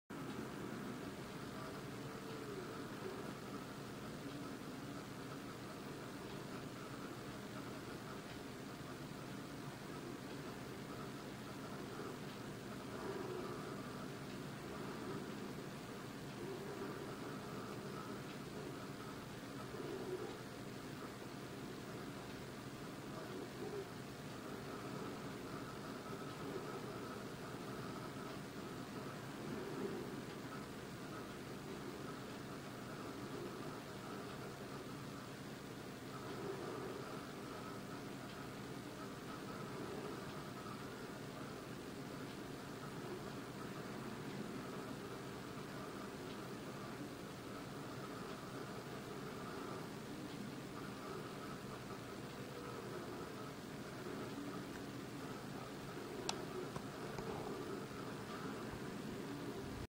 my_dragons.MOV
3 D objects, performed in Seamless 3 D, dragons by me, boat by Morning.star for Cybertown.com and fog animated by hypocrate